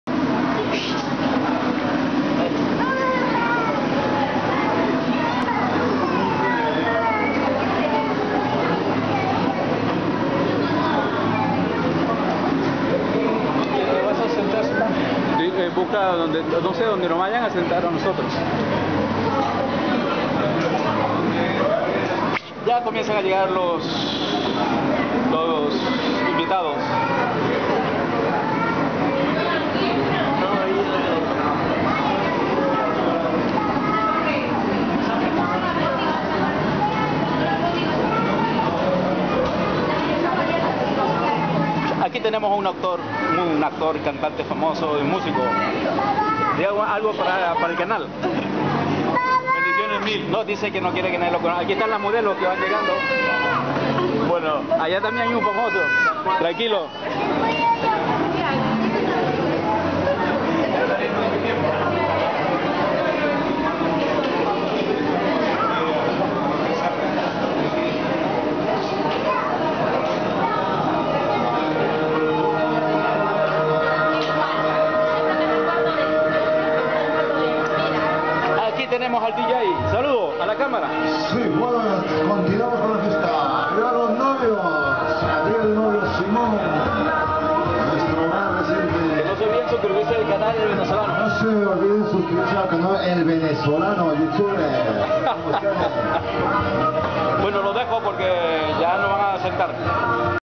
invitados llegando a la comida Madrid españa
bueno otro resumen rápido de la comidas en la celebración de la boda esperando a los invitados ,comienzan a llegar,pues la comida se ve estupenda.

hecha por una de las mejores chef cocineras del país jejej,nuestra amiga alexandra y compañía,la verdad fue un equipo en la cocina estupendo,una buena comida con sus canapés y etc ,todo de una boda,digna